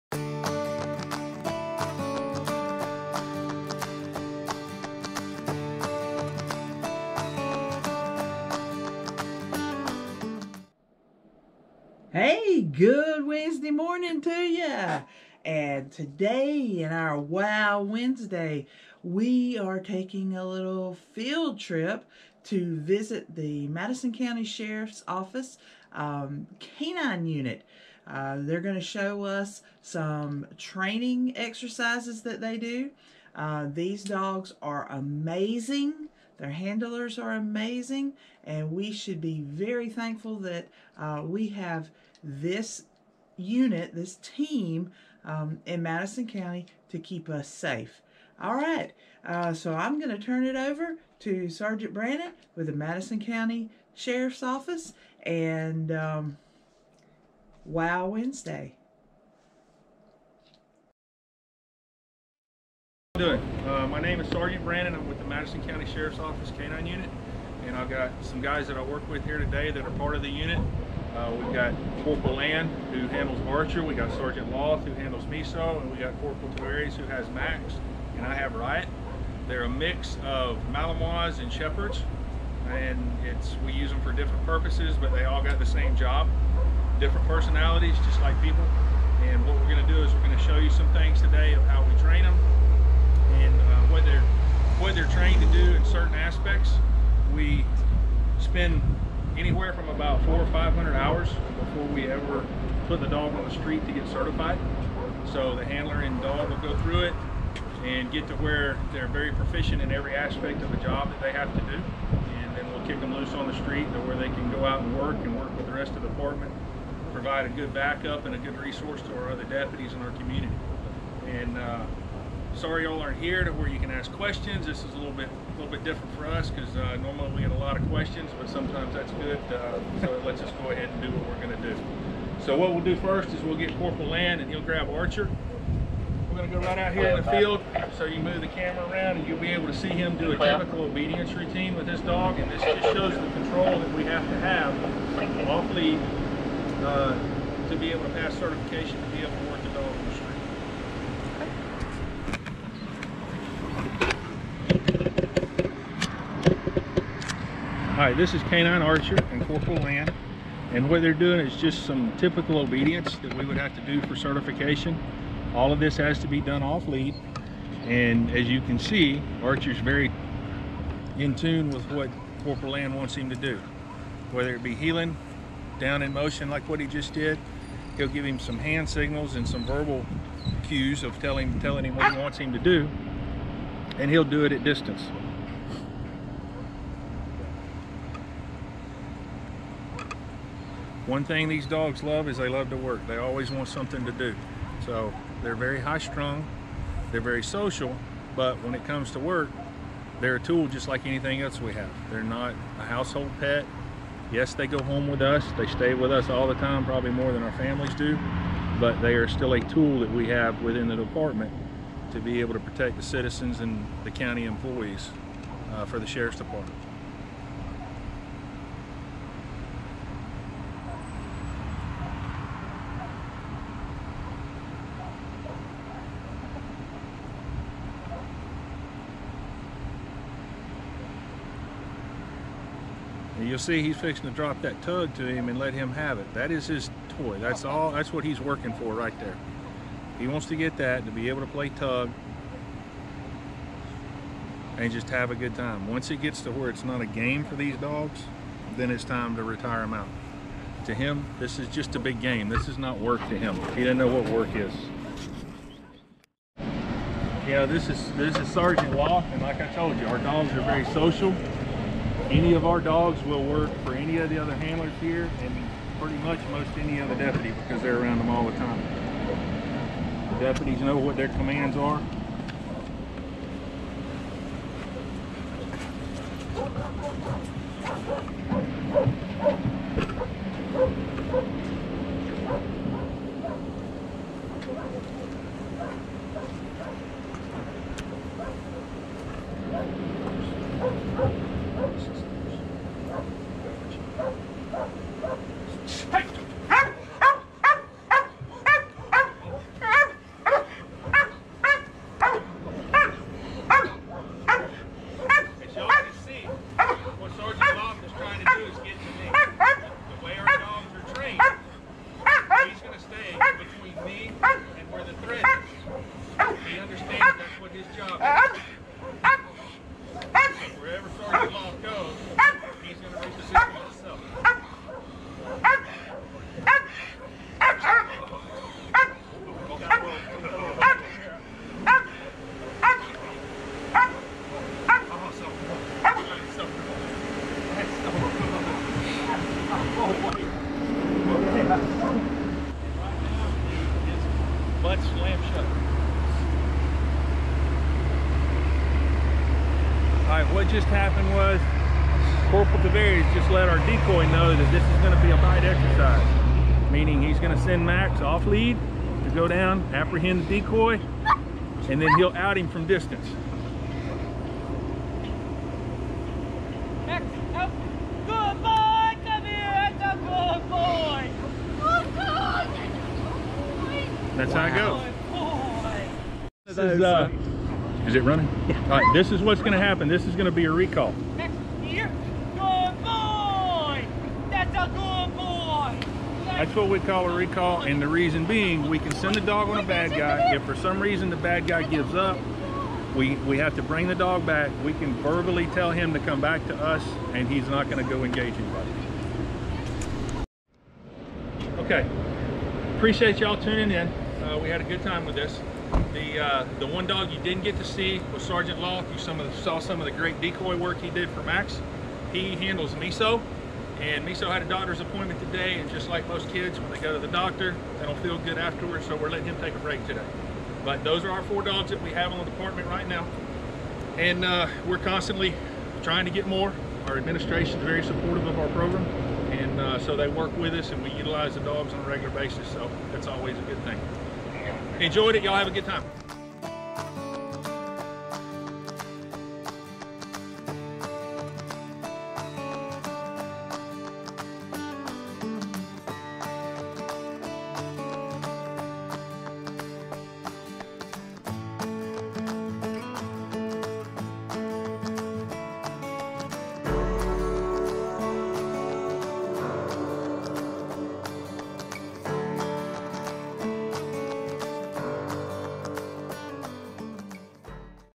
K 9 Unit
Learn about Madison County Sheriff's office K-9 unit.  These dogs are amazing.